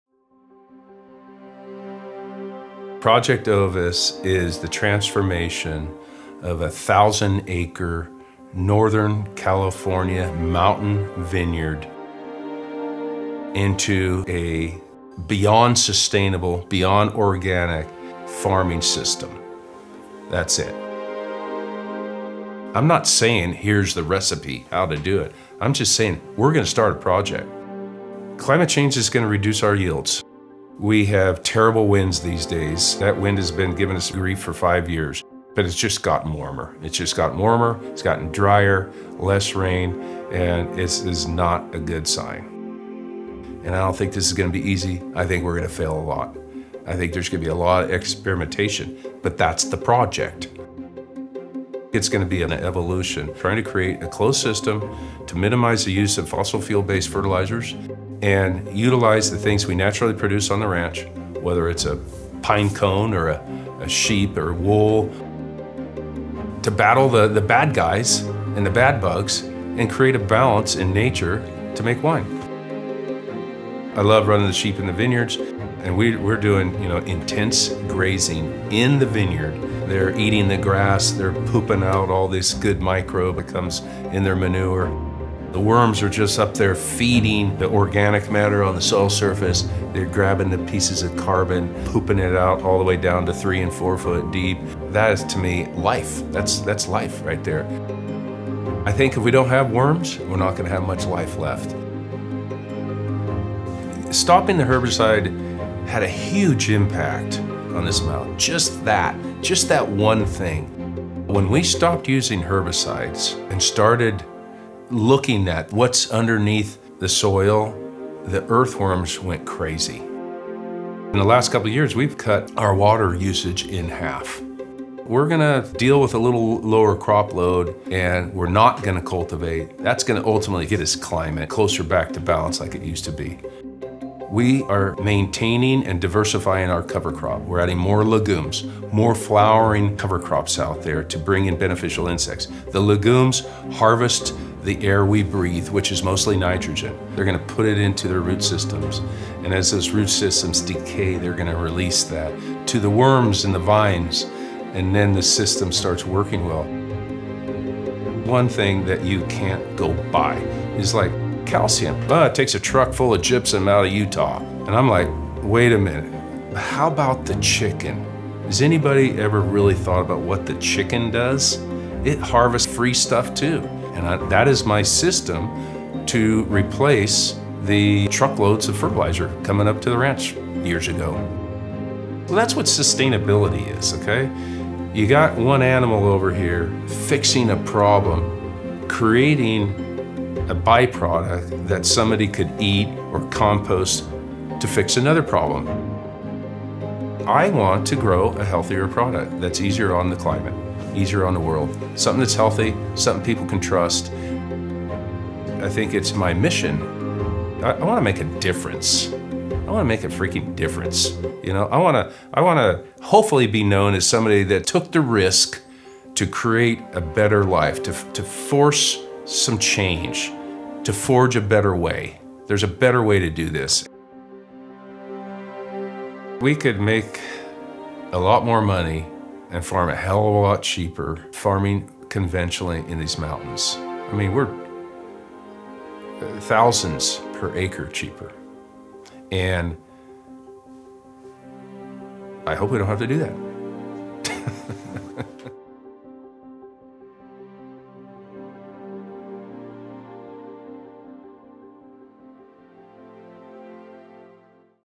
Clay Shannon's Project Ovis
Clay Shannon explains his "Project Ovis", a the transformation of a 1,000 acre Northern California mountain vineyard into a beyond sustainable, beyond organic farming system. We don't have all the answers, but we're starting a project. We're taking the risk to create a better life; to force some change; to forge a better way. The planet is the priority – not the bottom line.